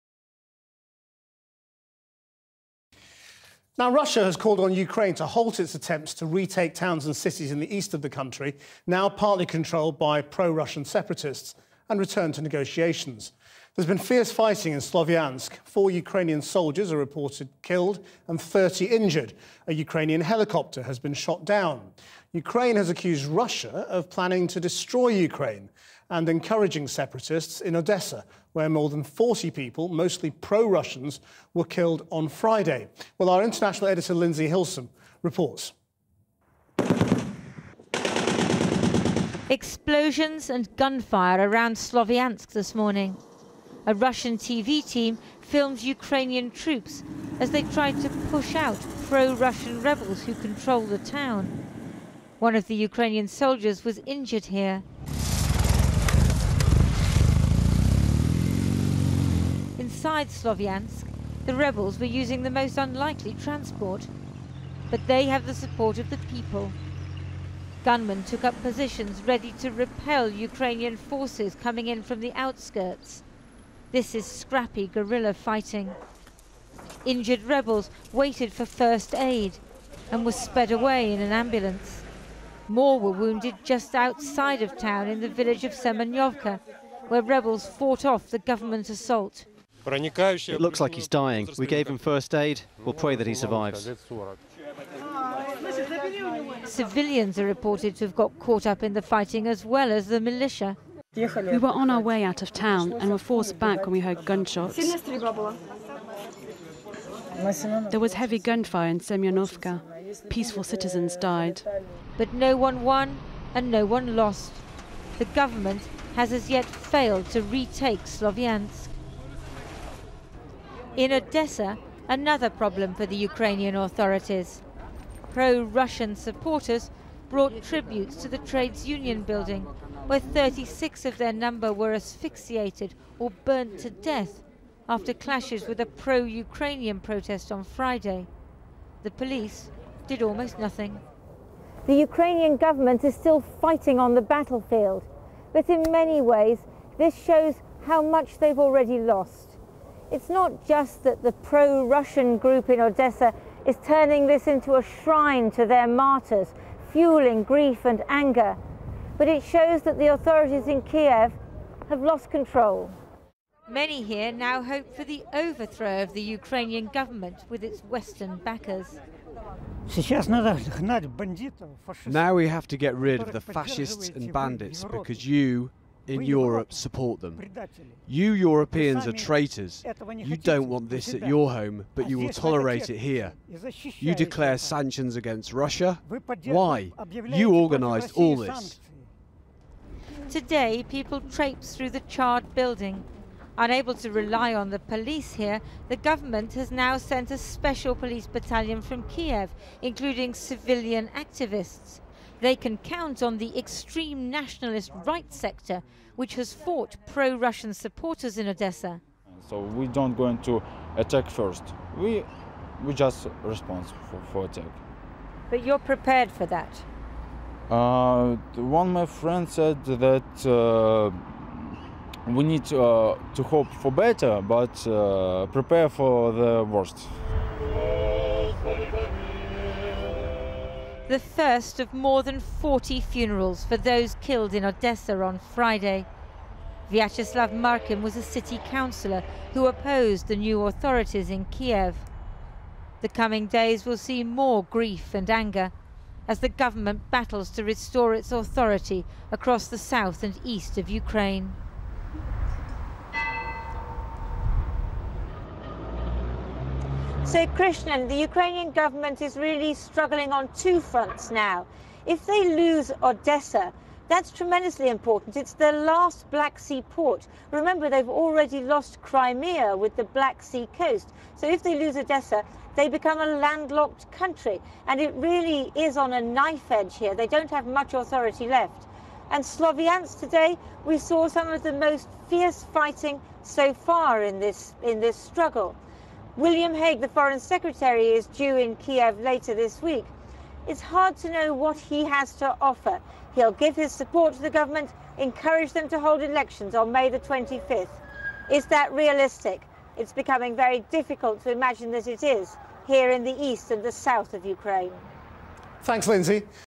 Soldiers die as Ukraine fights to regain control of east
Russia has called on Ukraine to halt its attempts to re-take towns and cities in the east of the country now partly controlled by pro-russian separatists. And return to negotiations.   .Sign up for Snowmail, your daily preview of what is on Channel 4 News, sent straight to your inbox,